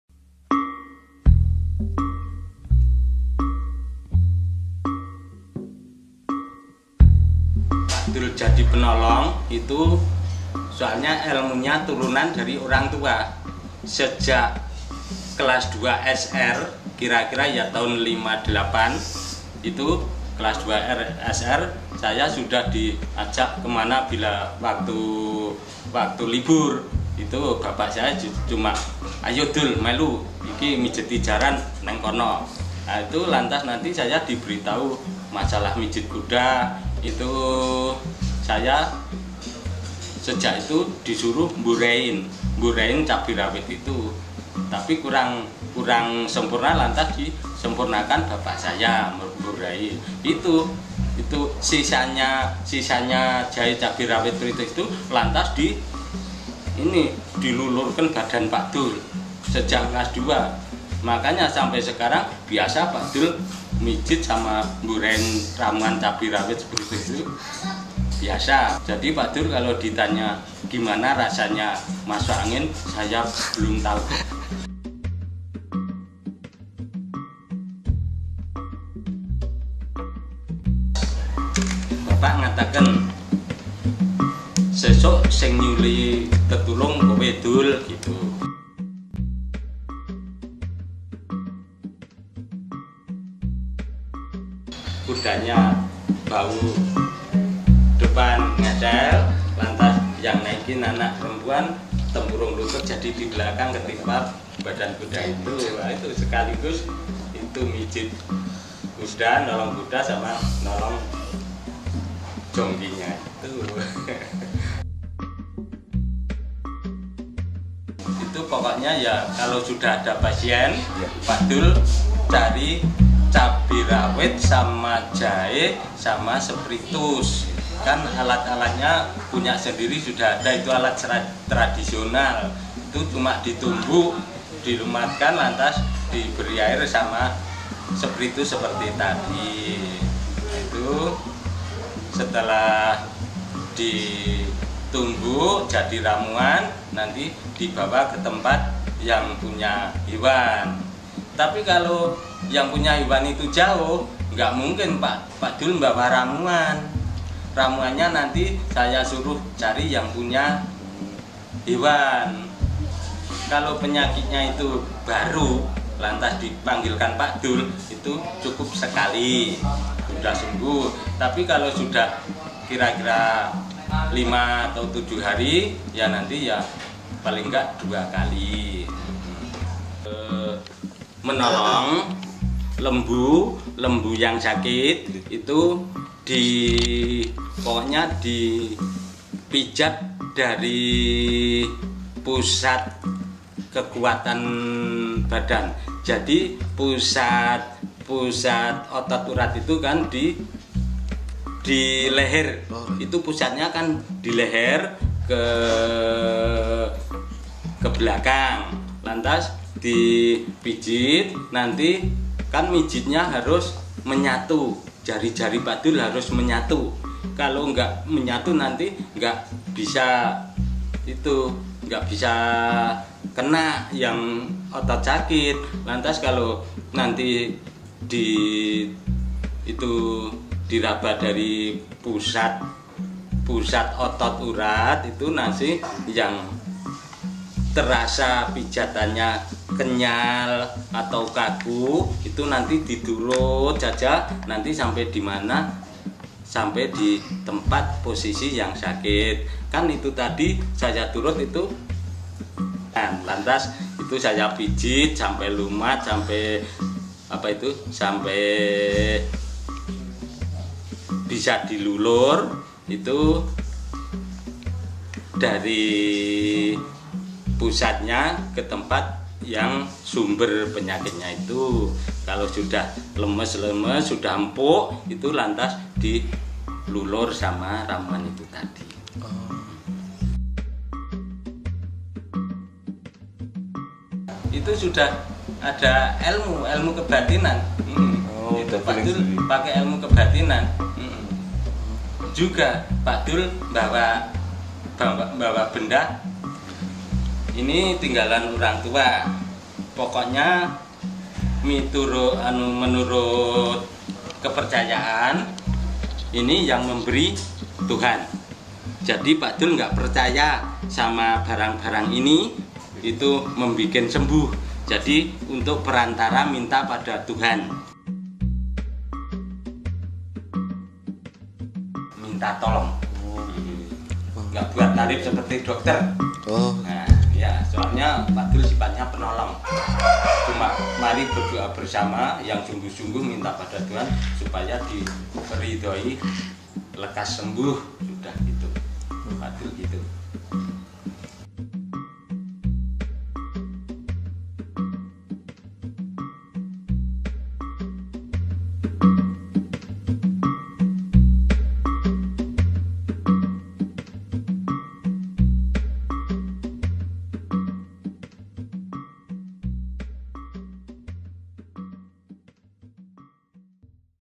Mbah Dul Sang Pemijat Sapi
Hanya manusia yang suka dipijat? Ternyata tidak. Sapi pun, untuk suatu keperluan perlu juga untuk dipijat. Bagaimana sapi ini dipijat dan bagaimana pula para pemijat ini belajar teknik memijat sapi? Temukan jawabannya dalam video berikut ini!